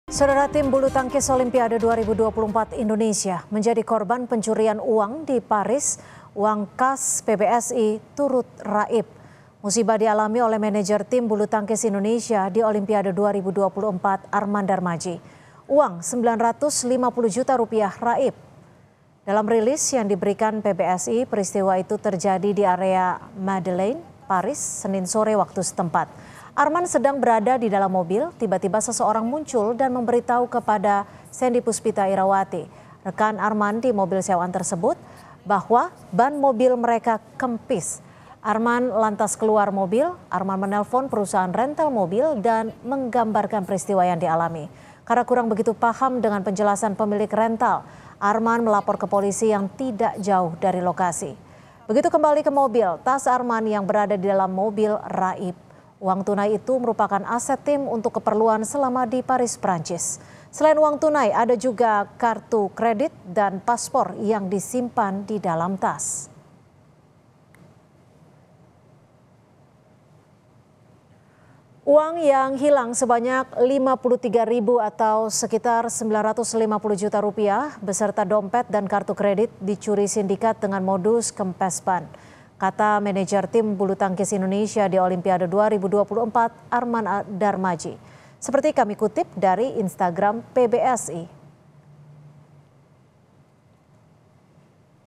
Tim Bulu Tangkis Olimpiade Asal Indonesia Kemalingan di Paris, Uang Rp 950 Juta Raib
PARIS, KOMPAS.TV - Tim Bulu Tangkis Olimpiade 2024 Indonesia menjadi korban pencurian uang di paris, uang kas pbsi turut raib.

Musibah dialami oleh Manajer Tim Bulu Tangkis Indonesia di Olimpiade 2024, Armand Darmadji. Uang Rp950 juta raib.

Dalam rilis yang diberikan PBSI, peristiwa itu terjadi di area Madeleine, Paris, Senin (05/08/2024) sore waktu setempat.

Armand sedang berada di dalam mobil. Tiba-tiba seseorang muncul dan memberi tahu kepada Shendy Puspita Irawati, Rekan Armand di mobil sewaan tersebut bahwa ban mobil mereka kempis, Armand lantas keluar mobil.

Baca Juga Menpora Dito Ariotedjo soal Persiapan Indonesia di Laga Olimpiade Paris 2024 di